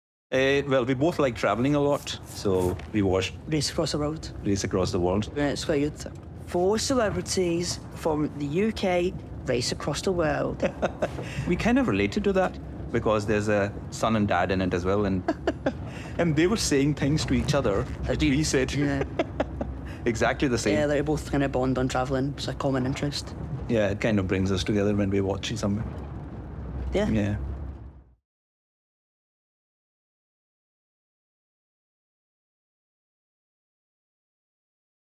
These dogs are already planning their next big trip 🌍 | Things We Love - BBC
Stressful travel on a tight budget? We can all relate to Race Across the World 🗺️

Real-life audience members share what they love about the BBC, with their voices and unscripted conversations matched to charming stop-motion clay characters.